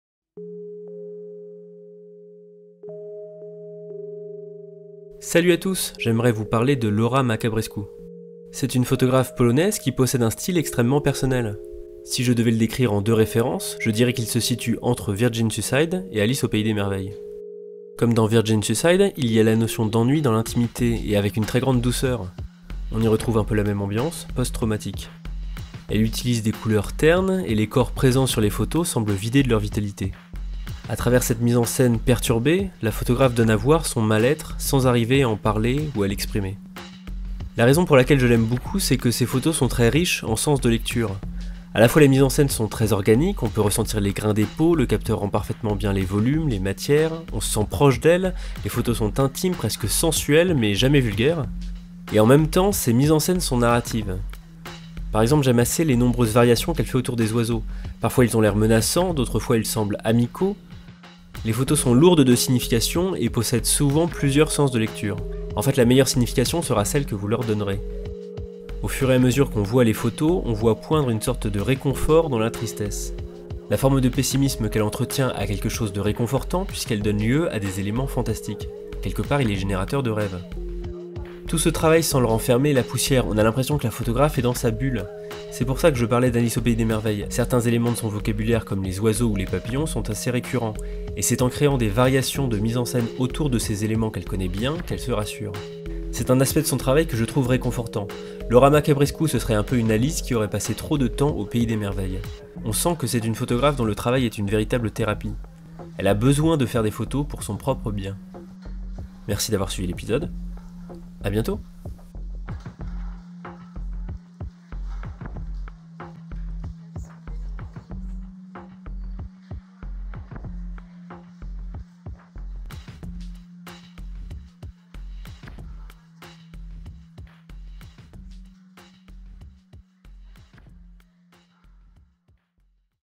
Laura Makabresku ⎸ PICS PILLS #14
Petite vidéo sur une photographe dépressive (nan mais ça faisait longtemps que j'avais pas fais un dépressif!)

Laura Makabresku c'est une photographe qui a aujourd'hui 30 ans (et ça fait encore mal à l'égo). Elle est polonaise, et je sais pas vous, mais avec toutes ces photographes de l'est que je suis en train de traiter, je trouve qu'il y a un certain point commun dans le traitement de l'identité. Il y a quelque chose de très pudique dans l'approche des corps (enfin très pudique, allez voir son flickr ya aussi des boobs). Mais dans son approche des images, je trouve que parfois elle ressemble un peu à Frida Kahlo parfois. Ses jeux avec les ficelles et les oiseaux, il y a quelque chose non?